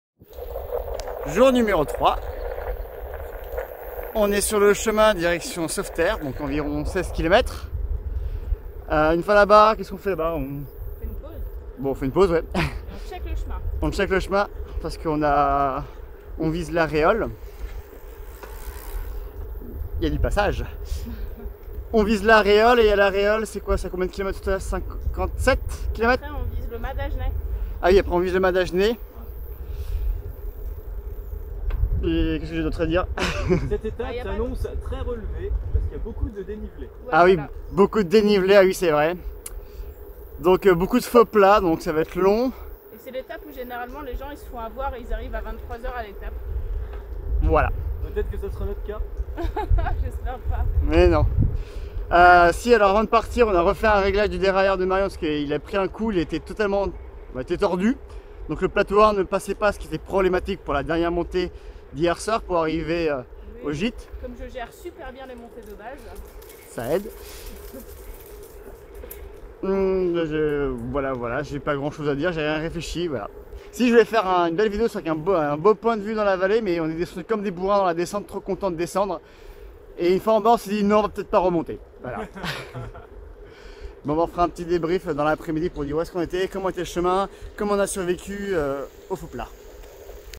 Le canal des deux mers en vélo, Jour 3 (1/5) Courpiac - Le Mas-d'Agenais : Départ
Départ direction Le Mas-d'Agenais.